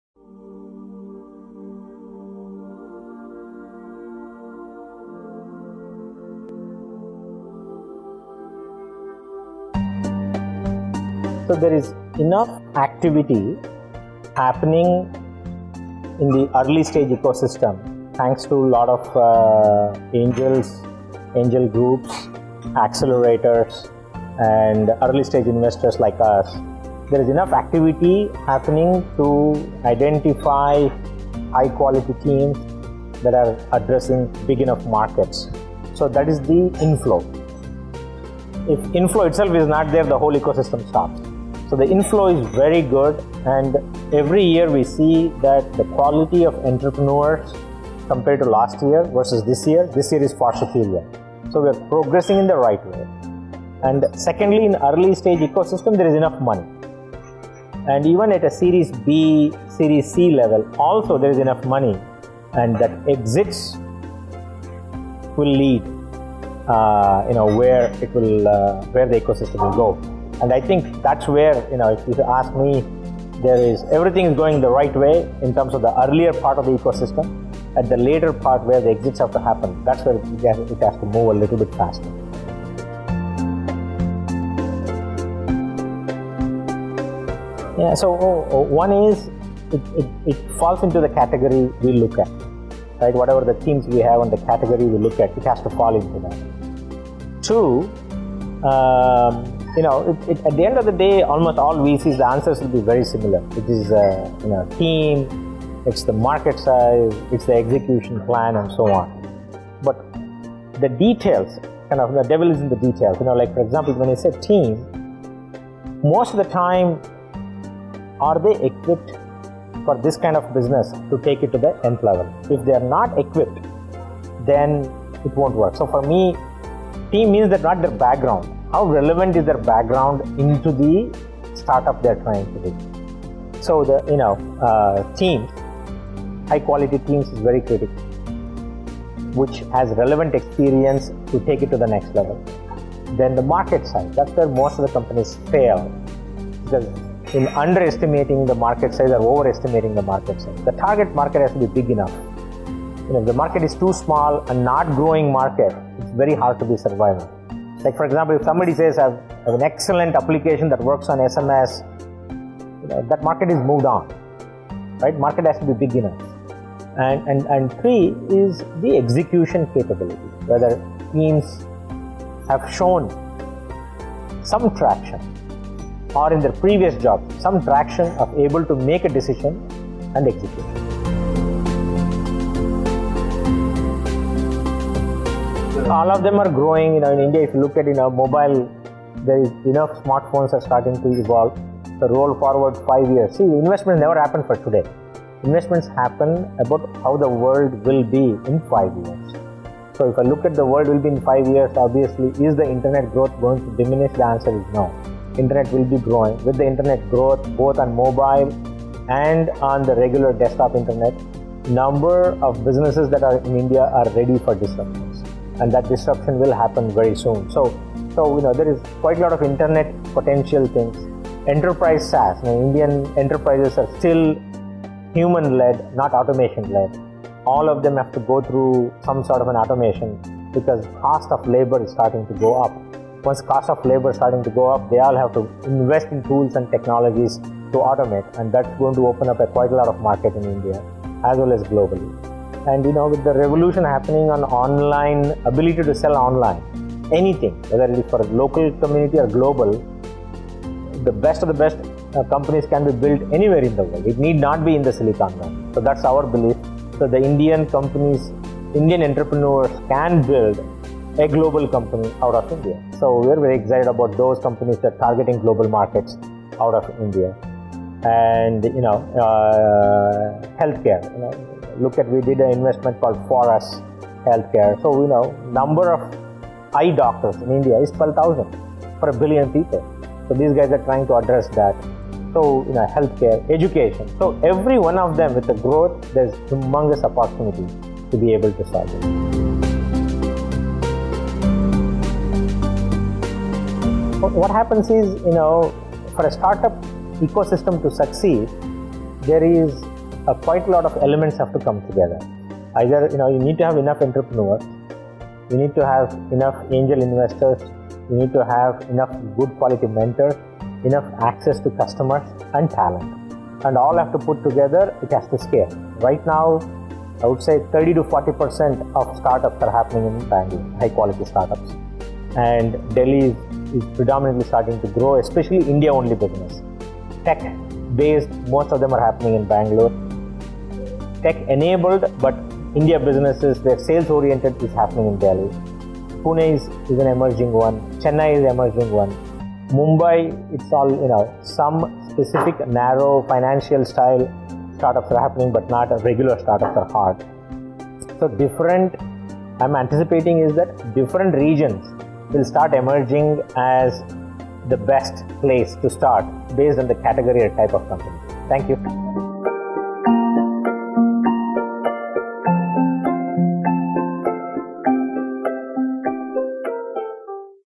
In conversation with Shekhar Kirani,Partner, Accel India
Hear Shekhar Kirani share his thoughts on Delhi as an emerging startup city and what aspects are required for the success of a startup.